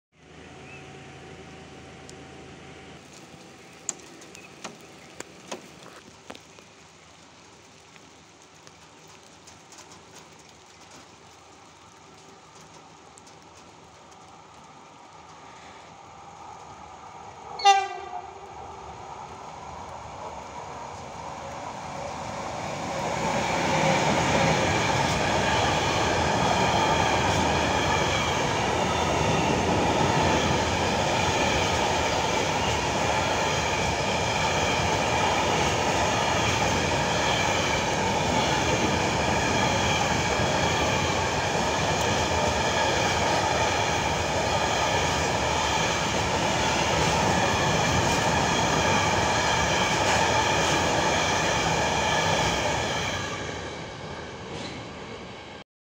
E186-926 z tadsami w stronę Czerwieńska + RP1 | Radnica
O POCIĄGU:
Nazwa: E186
Trasa: Rzepin  → Czerwieńsk
Wagony:kryte